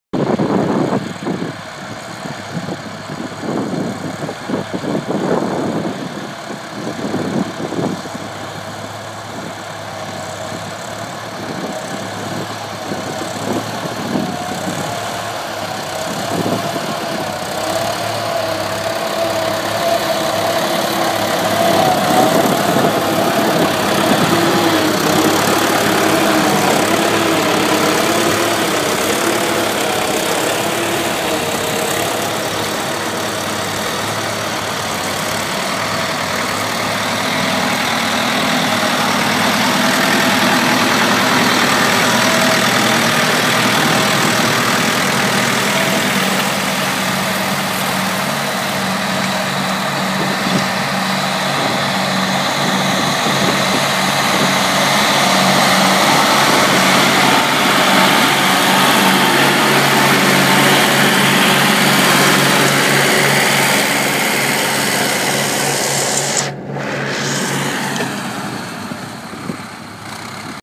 3 Doe Tractors ploughing at the Doe Show 7th February 2013
3 Doe Tractors ploughing at the Doe Show 7th February 2013 video taken during the workshop staff day out.